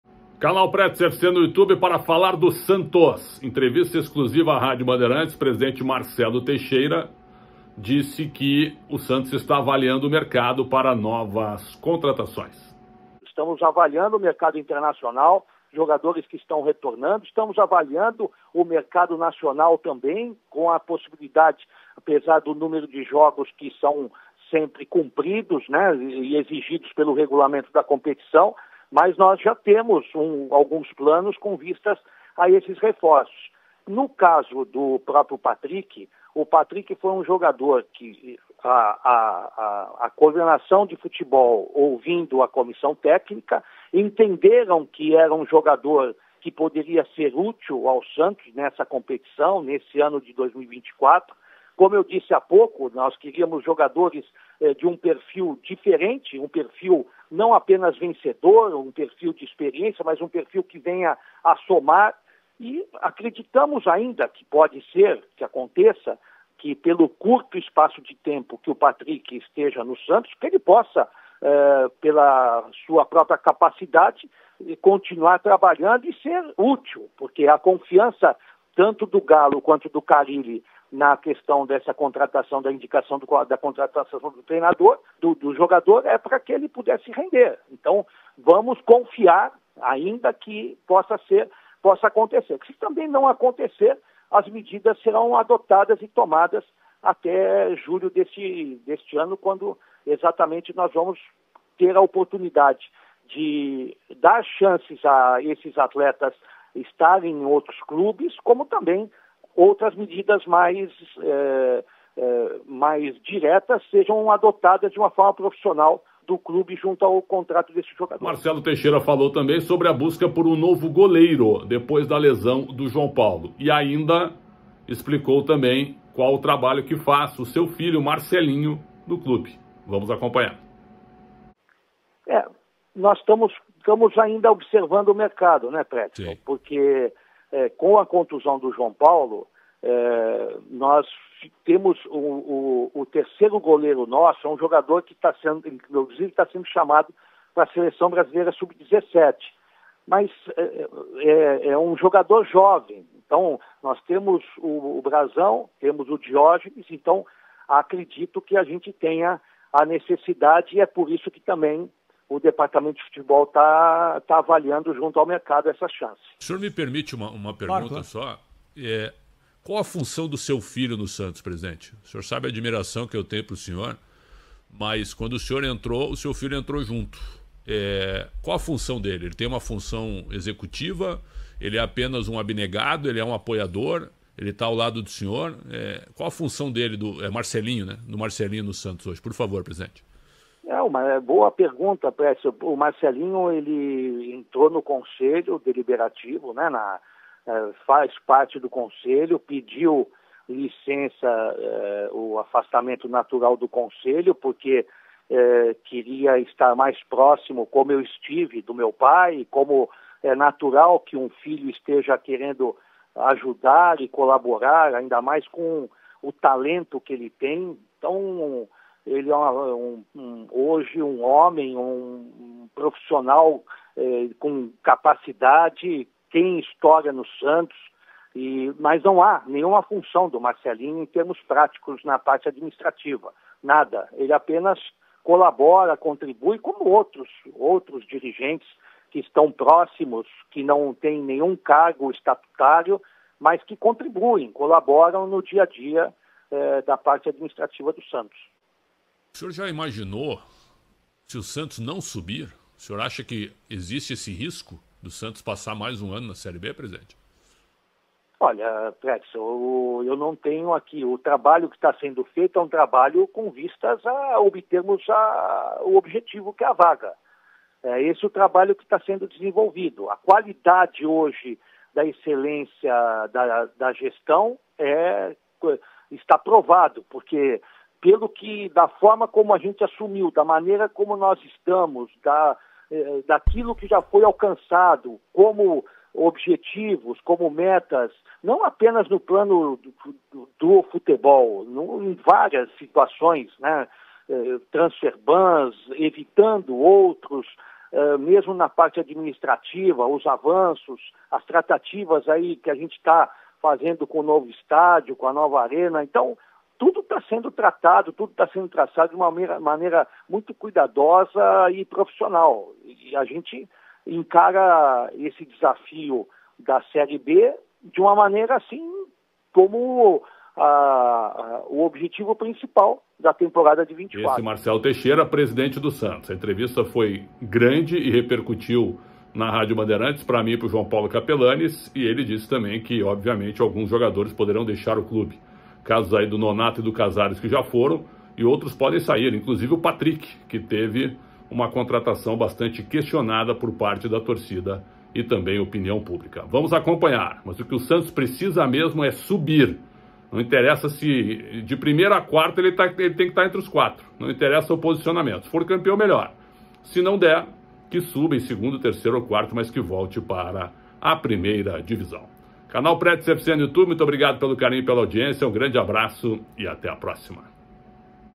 Marcelo Teixeira promete reforços e explica presença do filho na gestão do Clube
Santos vem de três derrotas consecutivas na Série B.